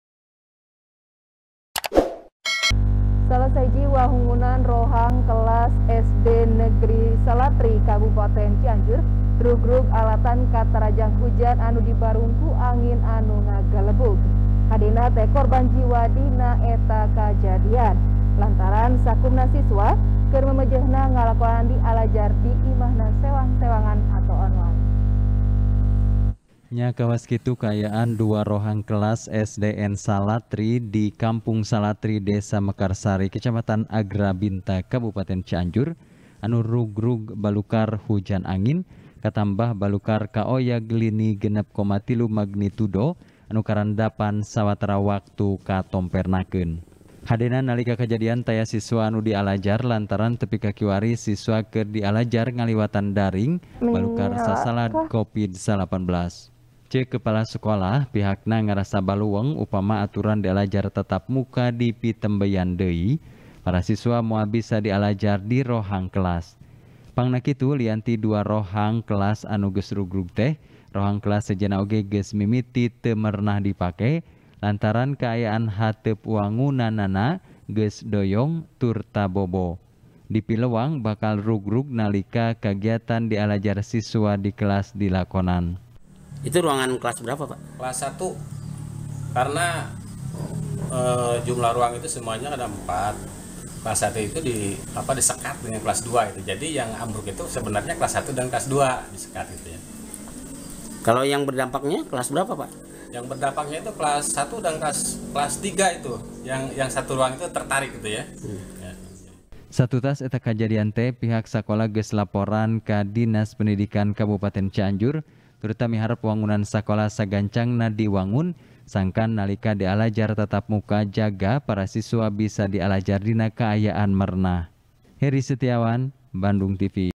Wangunan SDN Salatri Rugrug Balukar Hujan Angin | TP | BANDUNGTV
TP  | BANDUNGTV | ELBY NOPITRI | Salah Sahiji Wangunan Rohang Kelas Di Sekolah Dasar Negeri Salatri, Kabupaten Cianjur Rugrug Balukar Hujan Dibarung Angin Anu Ngagelebug. Hadena Taya Korban Jiwa Dina Eta Kajadian Teh, Lantaran Sakumna Siswa Keur Nyorang Dialajar Sacara Daring.